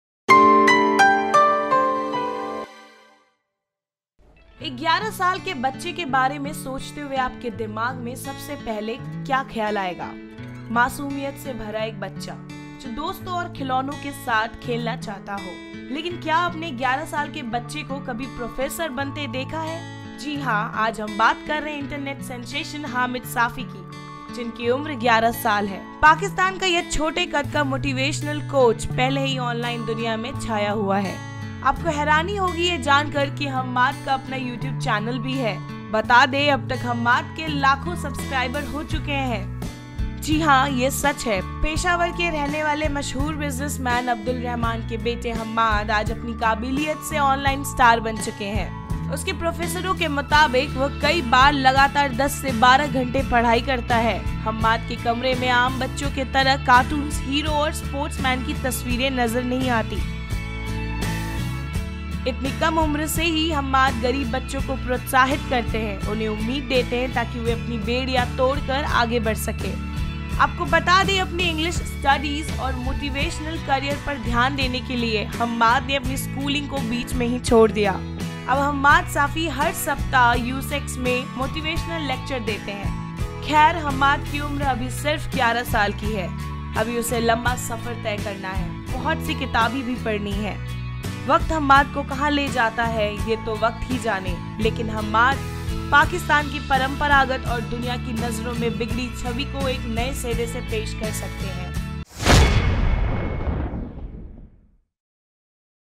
ऑनलाइन स्टार बन चुका 11 वर्ष का साफी USECS में मोटिवेशनल कोच || Newstimesindia
भोलापन,  अजीब बचपना, मासूमियत से भरा एक बच्चा... जो दोस्तों और खिलौनों के साथ खेलना चाहता है. लेकिन क्या आपने 11 साल के 'प्रोफेसर' के बारे में कभी सुना है?